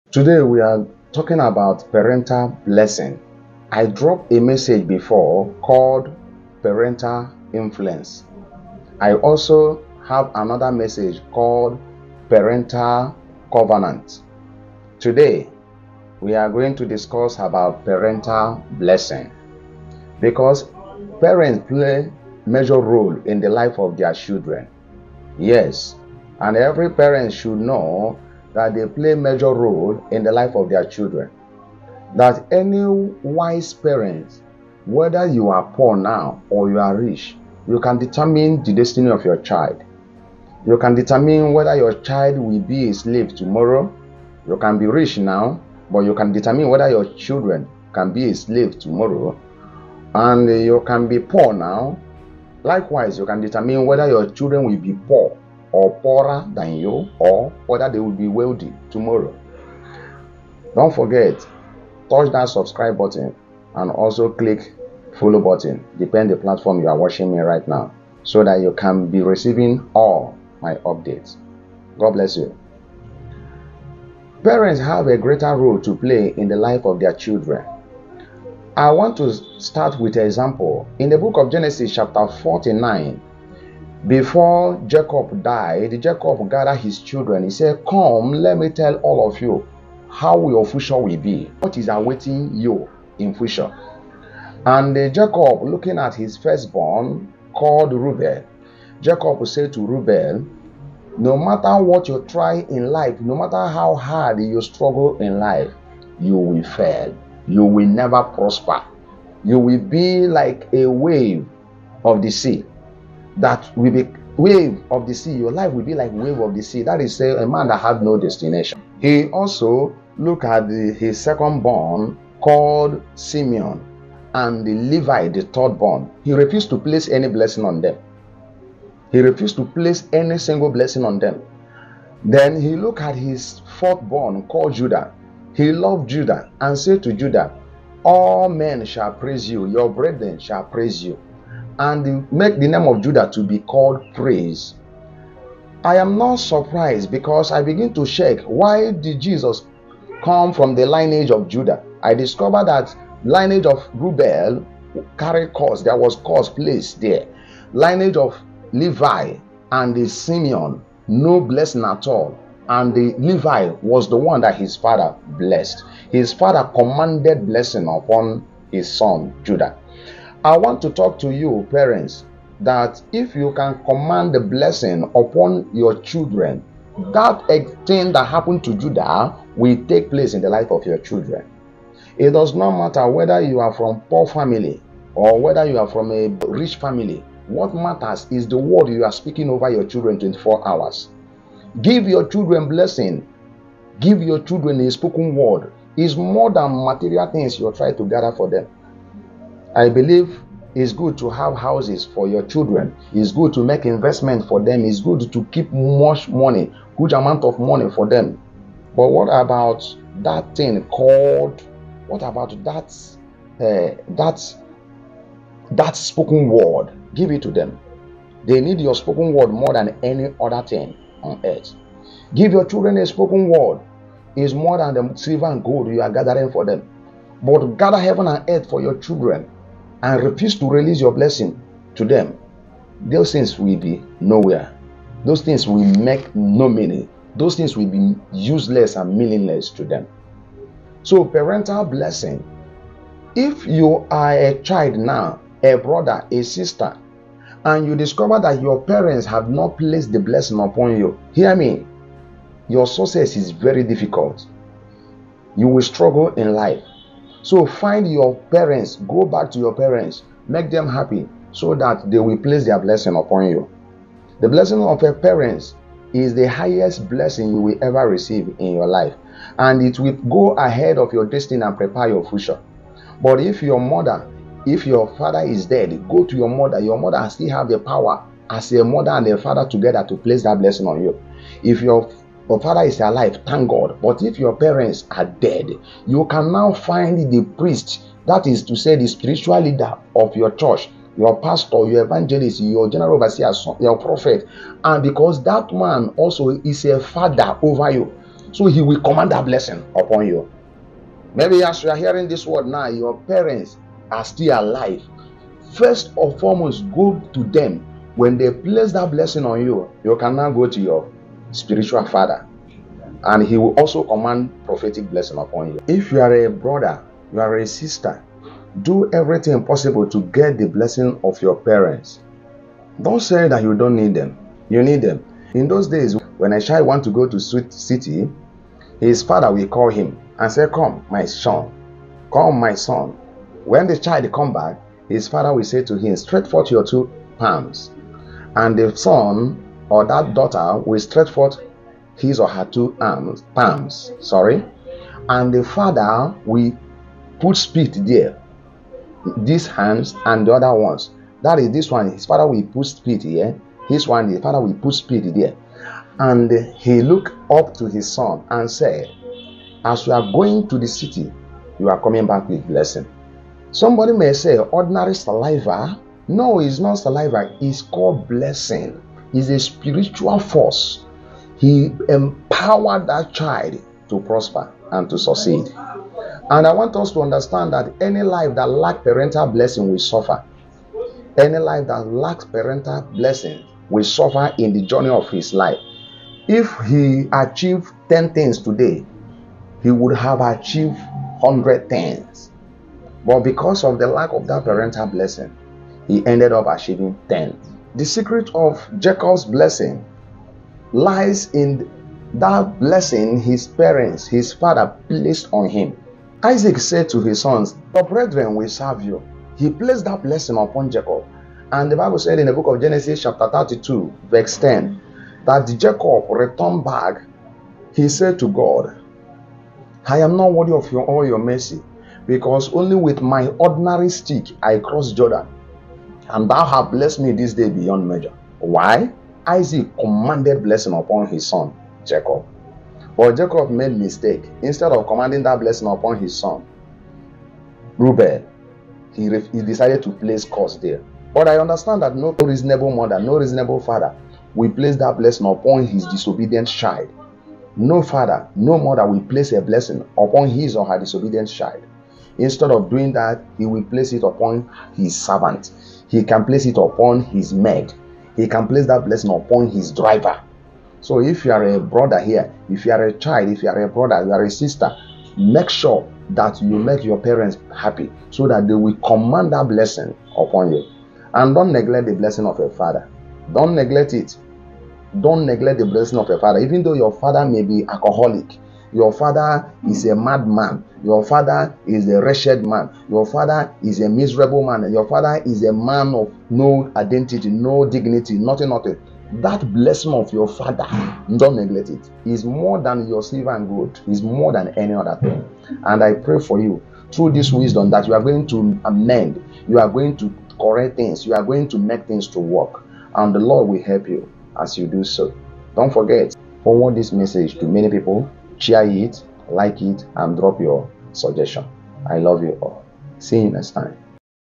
PARENT BLESSINGS
This video is talking about BLESSINGS FROM THE PARENT. Every parent can make their children so wealthy whether they the parents are wealthy or not through a spoken word of blessing over their children. The destiny/future of children lies in the hands of their parents especially FATHERS. make sure you get your children. Parents please please BLESSING upon your children.